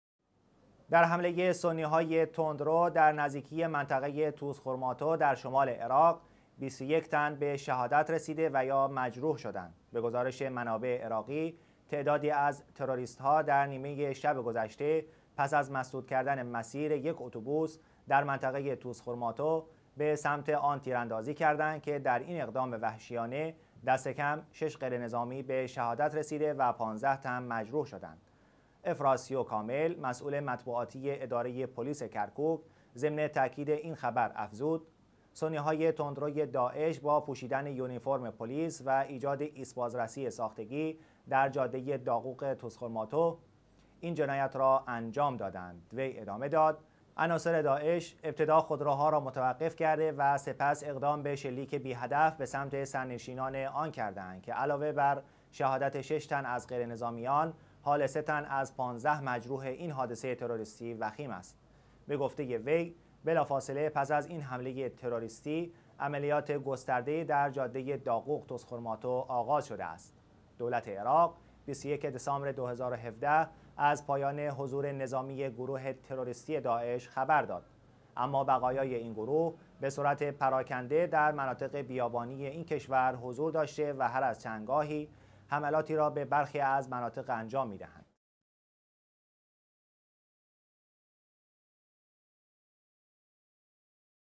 حمله تروريستي در جاده داقوق ـ طوز خورماتو عراق
در حمله سنی های تندرو در نزديکی منطقه طوزخورماتو در شمال عراق، 21 تن به شهادت رسیده و یا مجروح شدند.

به گزارش منابع عراقی، تعدادی از تروریست ها در( نیمه شب گذشته) پس از مسدود کردن مسير يک اتوبوس در منطقه طوز خورماتو، به سمت آن تيراندازي کرده اند که در این اقدام وحشیانه دستکم 6 غیر نظامی به شهادت رسیده و 15 تن مجروح شدند.

افراسیاو کامل، مسئول مطبوعاتی اداره پلیس کرکوک، ضمن تایید این خبر افزود: سنی های تندروی داعش با پوشیدن یونیفورم پلیس و ایجاد ایست بازرسی ساختگی در جاده داقوق ـ طوزخورماتو این جنایت را انجام داده اند.

وی ادامه داد: عناصر داعش ابتدا خودروها را متوقف کرده و سپس اقدام به شلیک بی هدف به سمت سرنشینان آن کرده اند که علاوه بر شهادت 6 تن از غیر نظامیان، حال 3 تن از 15 مجروح این حادثه تروریستی وخیم است.

به گفته وی بلافاصله پس از این حمله تروریستی، عملیات گسترده ای در جاده داقوق ـ طوزخورماتو آغاز شده است.

از سویی دیگر، «حمد نامس الجبوري» فرمانده پليس نينوا گفت: در پي یک عمليات امنيتي پيش گيرانه پليس نينوا در کرانه غربي موصل 20 نفر از عناصر داعش در منطقه سوق المعاش را بازداشت کرد.

سازمان مبارزه با تروريسم عراق هم طي عملياتي درعمق صحراي الانبار (غرب عراق) شماري از سنی های تندروی وابسته به گروه تروريستی داعش را بازداشت نموده است.

دولت عراق 21 دسامبر 2017 از پايان حضور نظامي گروه تروريستي داعش خبر داد، اما بقاياي اين گروه به صورت پراکنده در مناطق بیابانی این کشور حضور داشته و هر از چندگاهي حملاتي را به برخي از مناطق انجام مي دهند.